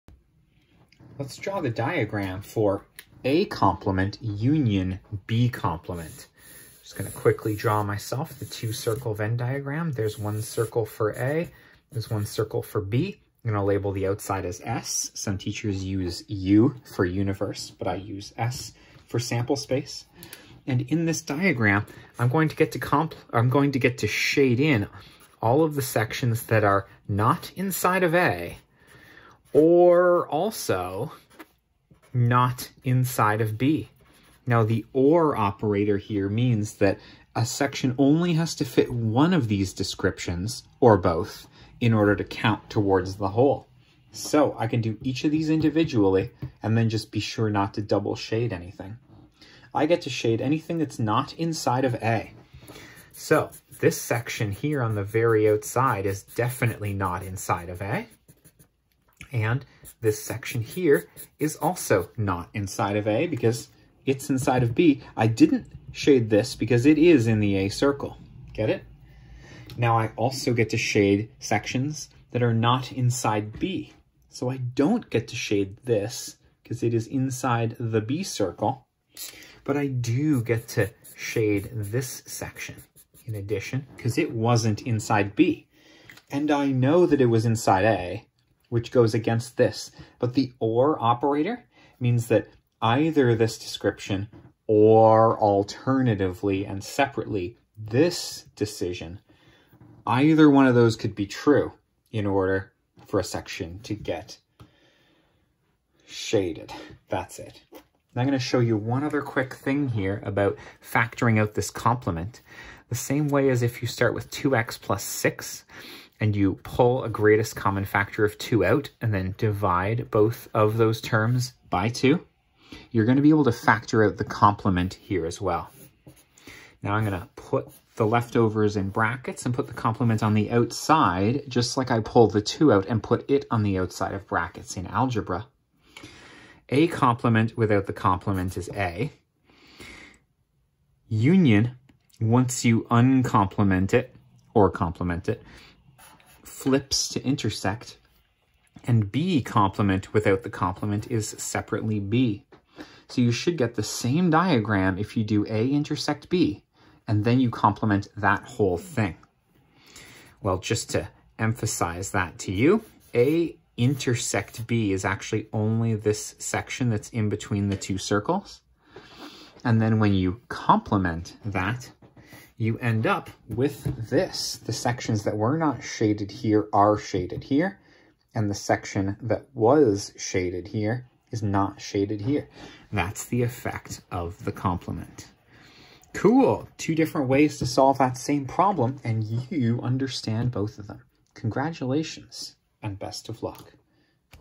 Venn Diagram of A' U B'
A' is all of the things that's AREN'T A
B' is all of the things that AREN'T B
A 'counts' because it's Not B
B 'counts' because it's not A
The only part that doesn't apply is A-intersect-B